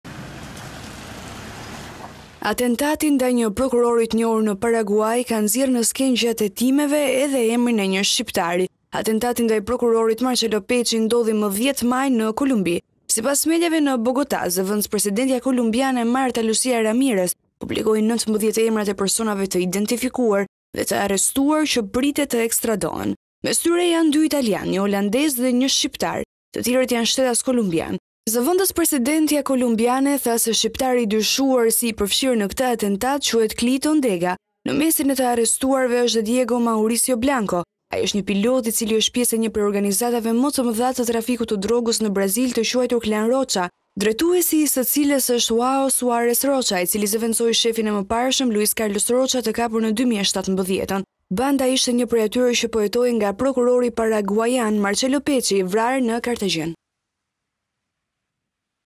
Top News- Shqiptari, atentat ndaj prokurorit/Bashkë me mafian kolumbiane, vrau Marcelo Pecci
Atentati ndaj një prokurori të njohur në Paraguai ka nxjerrë në skenë gjatë hetimve edhe emrin një shqiptari.
Atentati ndaj prokurorit Marcelo Pecci ndodhi më 10 maj në Kolumbi. Sipas mediave në Bogota, zv/presidentja kolumbiane, Marta Lucía Ramírez, publikoi 19 emrat e personave të identifikuar dhe të arrestuar që pritet të ekstradohen.
Mes tyre janë dy italianë, një holandez dhe një shqiptar. Të tjerët janë shtetas kolumbianë.
Zv.presidentja kolumbiane tha se shqiptari i dyshuar si i përfshirë në këtë atentat quhet Kliton Dega.
Në mesi e të arrestuarve është edhe Diego Mauricio Blanco Blanco. Ai është një pilot, i cili është pjesë e një prej organizatave më të mëdha të trafikut të drogës në Brazil të quajtur ‘Clan Rocha’, drejtuesi i së cilës është Joao Suarez Rocha, i cili zëvendësoi shefin e mëparshëm, Luis Carlos Rocha, të kapur në 2017.
Banda ishte një prej atyre që po hetohej nga prokurori paraguaian Marcelo Pecci, i vrarë në Kartagjenë.

Pamje:AFP

Photo By BogotaPost